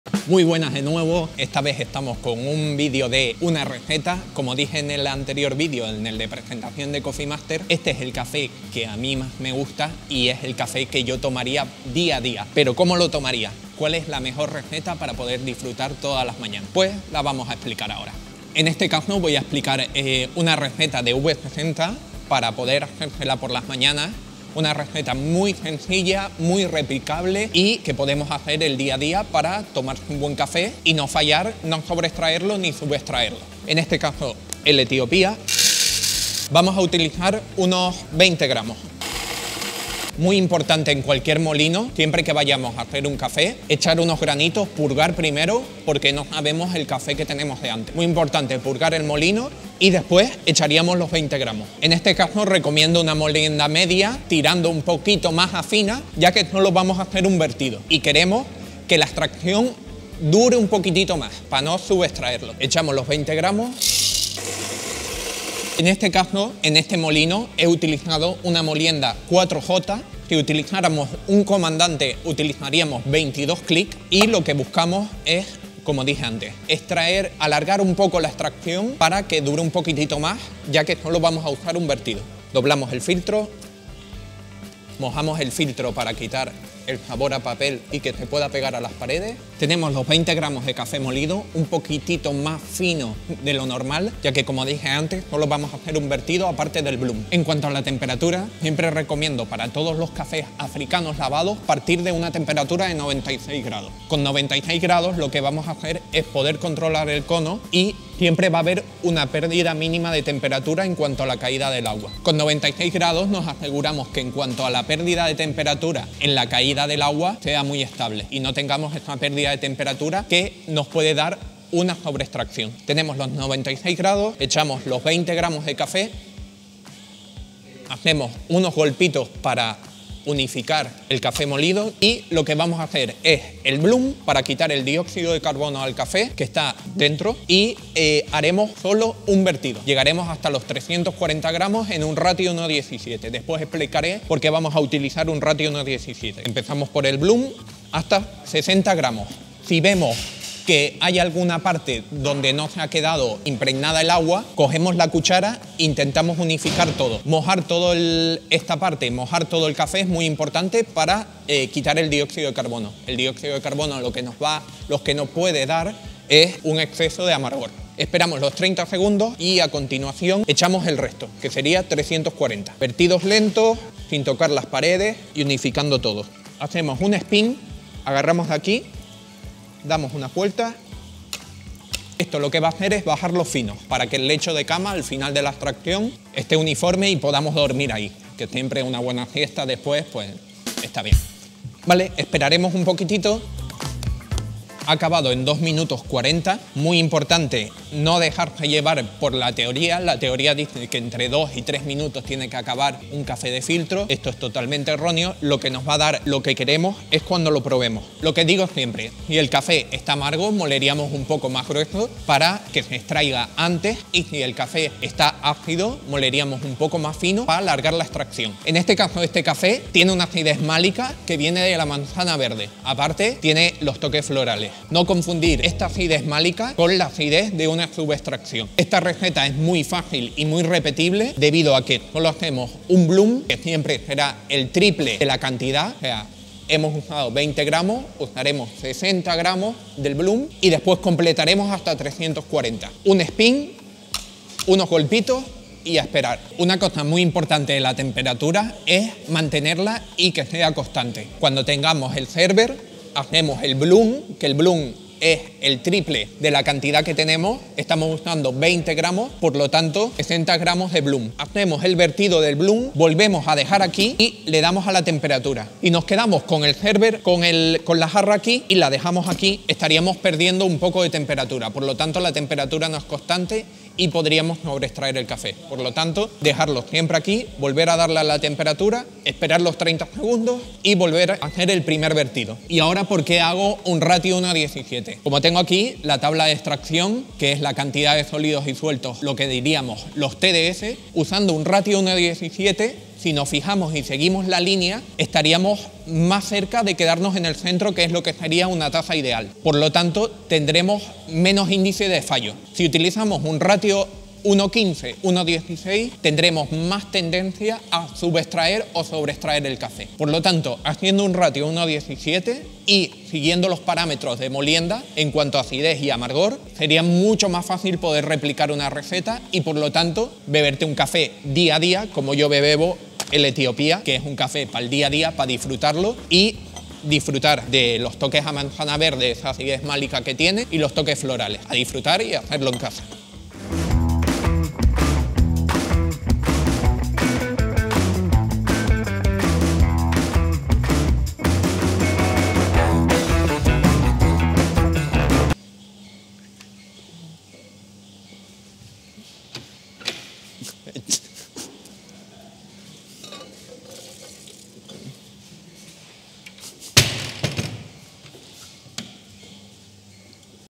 RECETA V60 con un café de COFFEE MASTERS
🔥 ¡Aquí tenemos a Adri contando cómo hacer un cafelazo de la suscripción Coffee Masters en V60!

Si quieres formar parte de la Sociedad Secreta de Coffee Masters